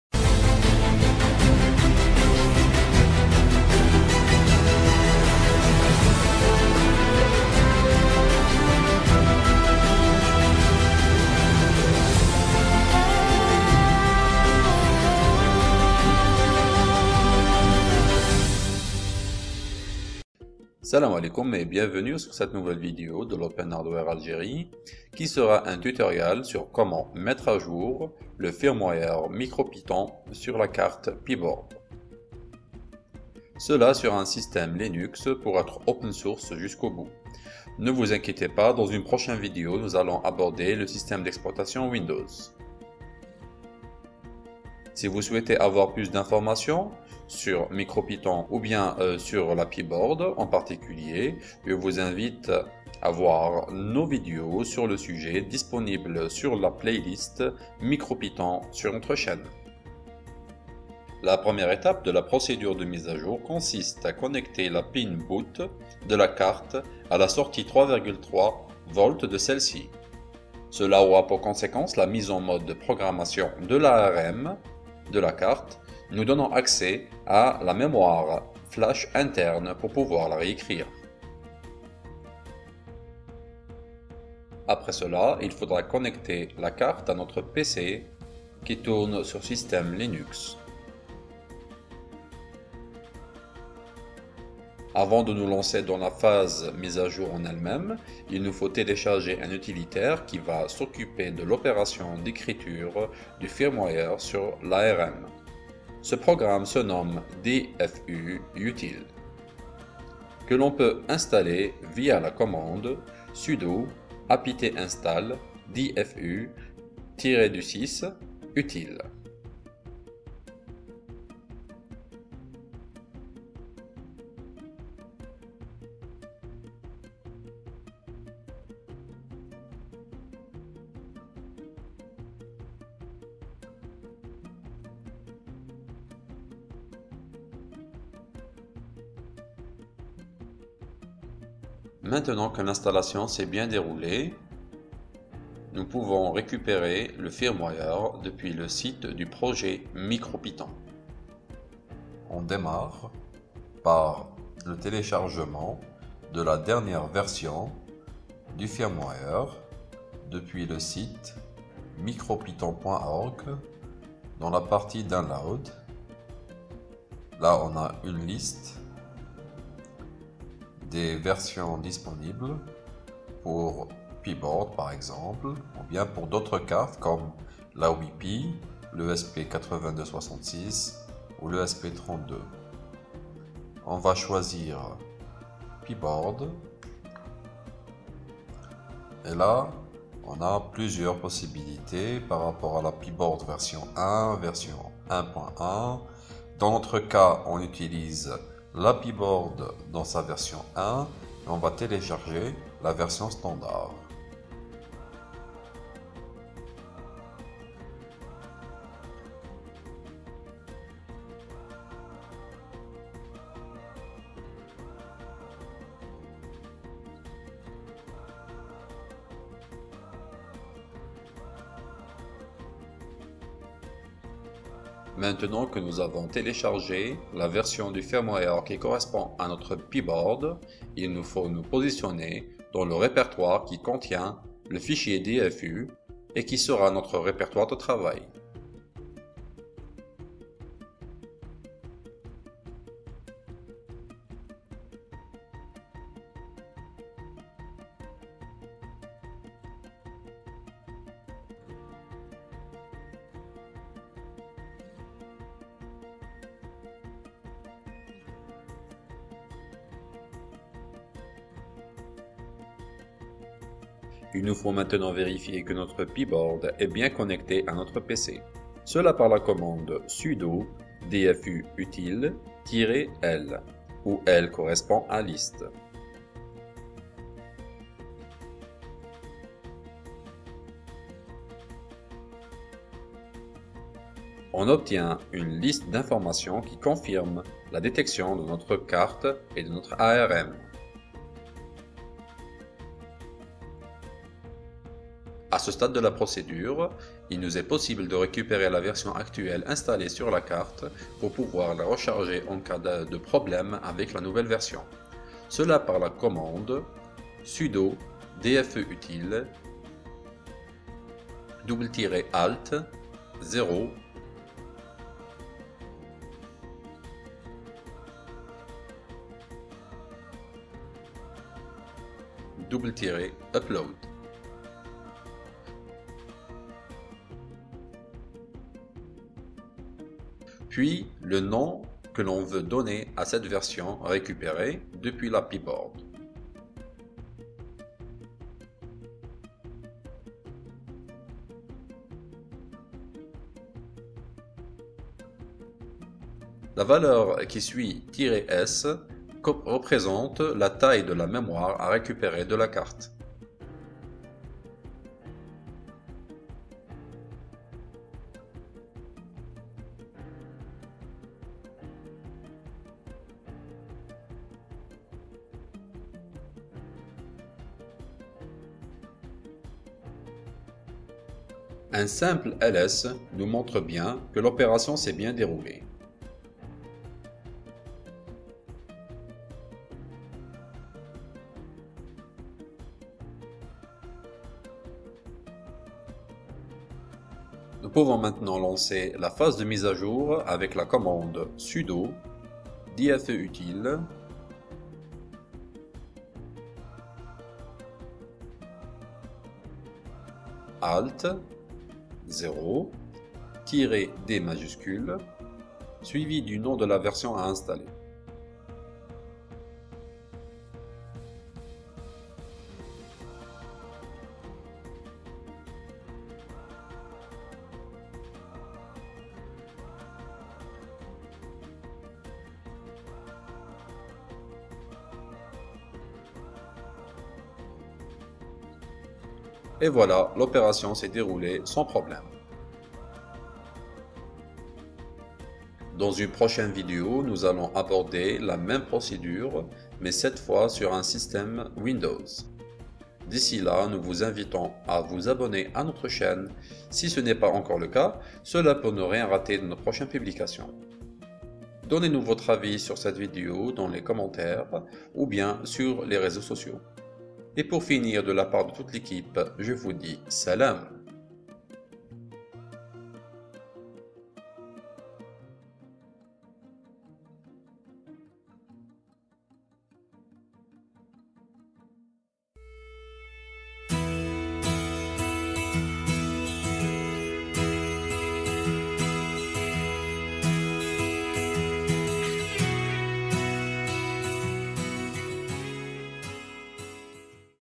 M-à-j du firmware MicroPython de la Pyboard sur le système Ubuntu Linux
Dans cette vidéo tutoriel nous vous expliquons comment mettre à jour le firmware MicroPython de votre carte pyboard.

le lien de téléchargement des mise à jours des carte qui supportes MicroPython: